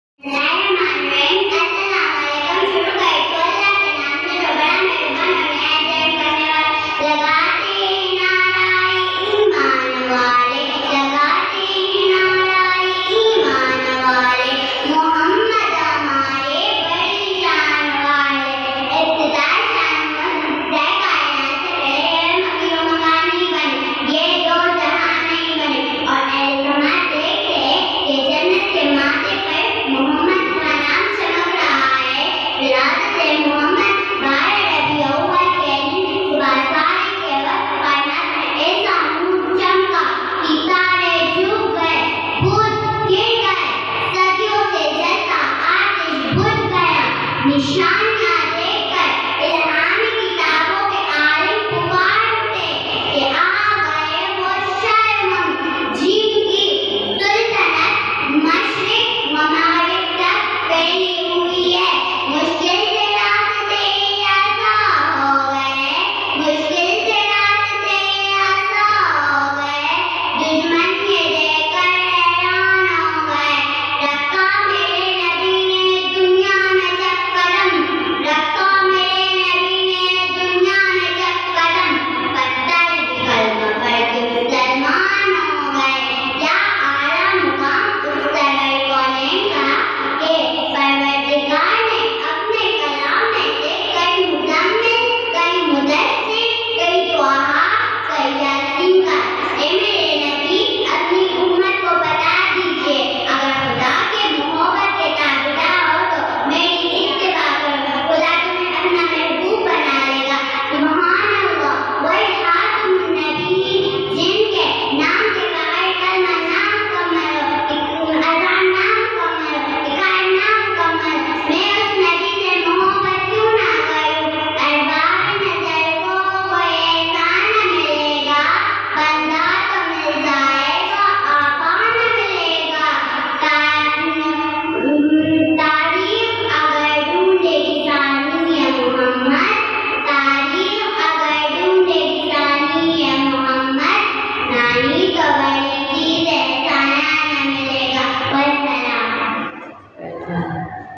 A lovely little girl's speech on the occasion of Eid Milad-ul-Nabi Dur E Marjan k.g orange
Speech on the occasion of Eid Milad-ul-Nabi

عید میلاد النبی کے موقع پر ایک پیاری سی بچی کی تقریر